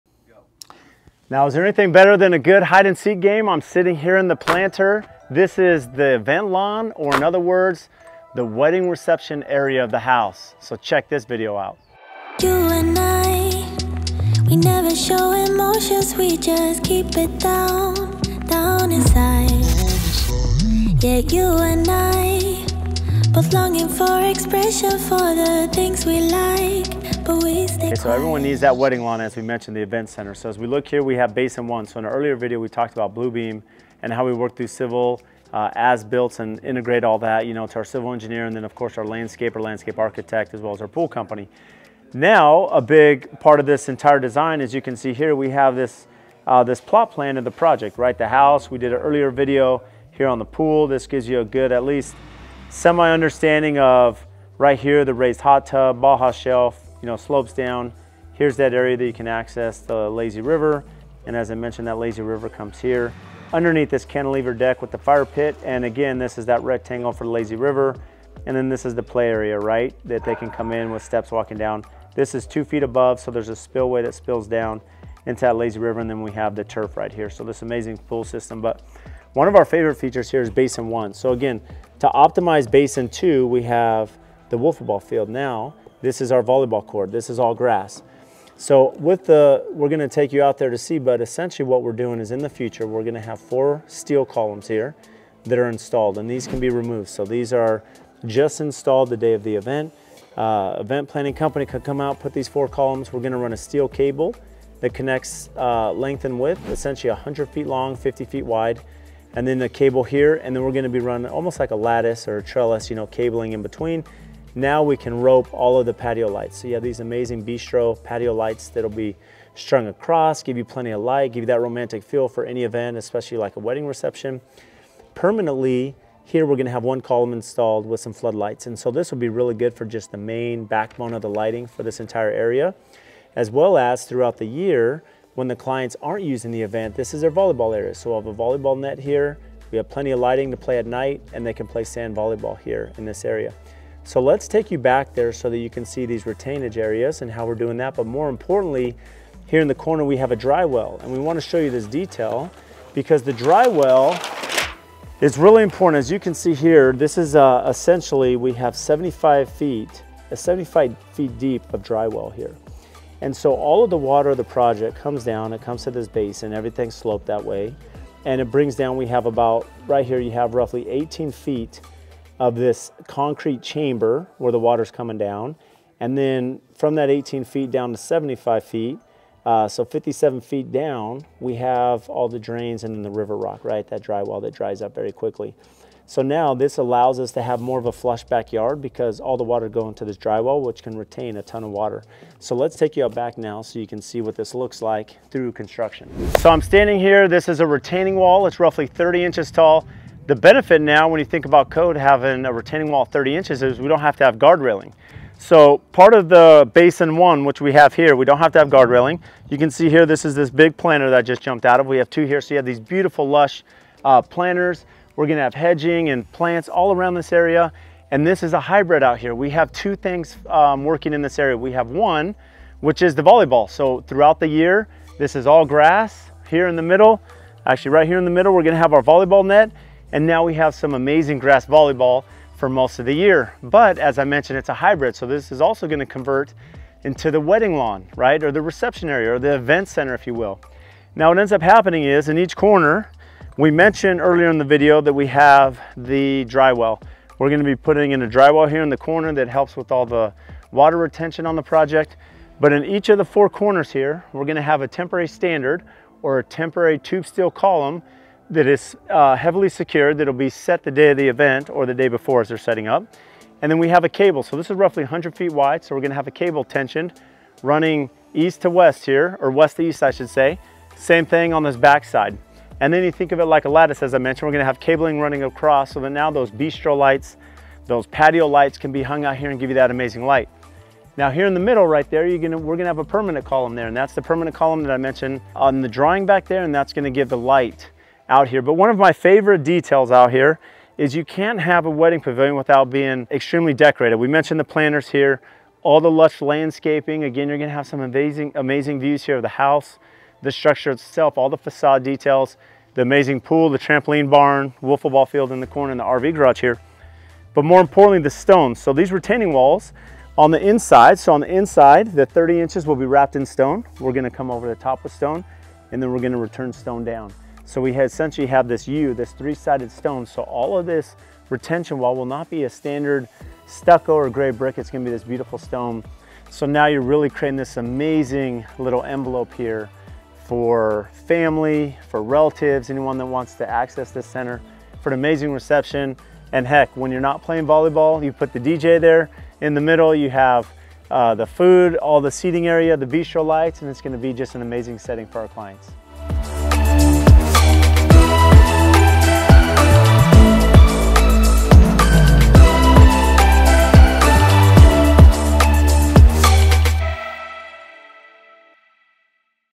The Wedding Lawn | Multi-Purpose Space | Project Blow Your Mind | AFT Construction
In this episode, Brad talks about the wedding lawn at our Project Blow Your Mind. This multi-purpose space is the event lawn but also retains all the water for this area of the yard. Check out the dry-well and what this space will do.